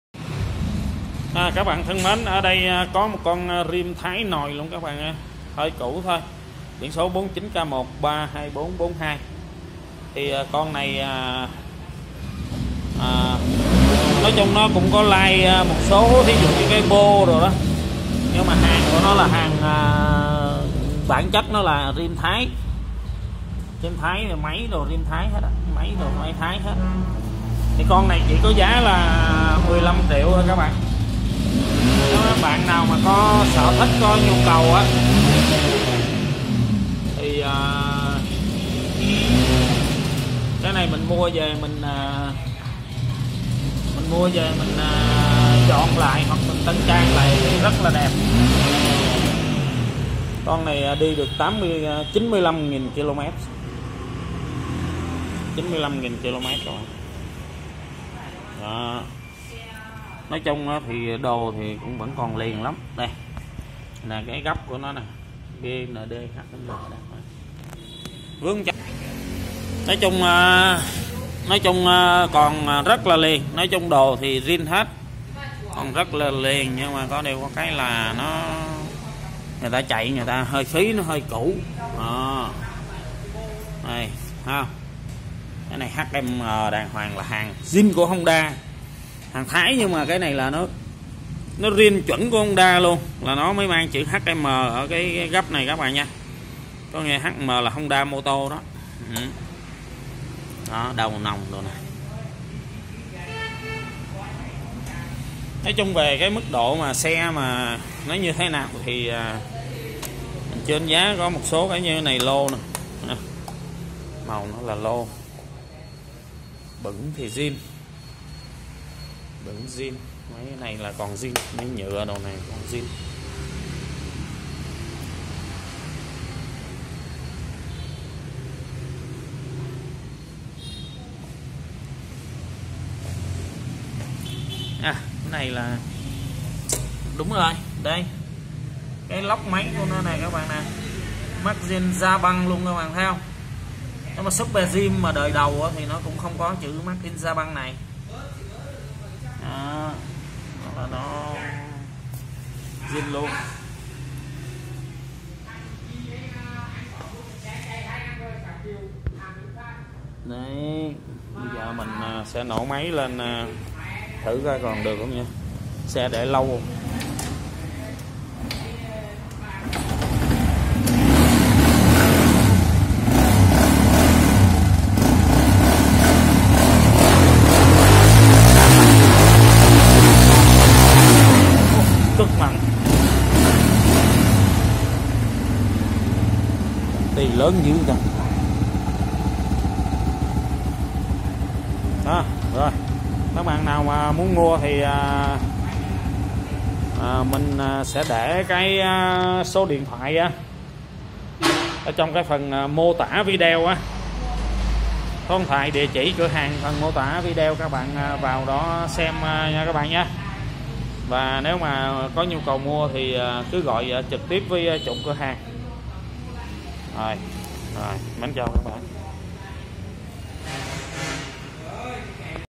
Bán xe honda dream thái zin giá 15 triệu gọi 0915365468.
Cửa hàng xe máy Hoàng Dương.
ĐC: thị trấn đinh văn, lâm hà lâm đồng.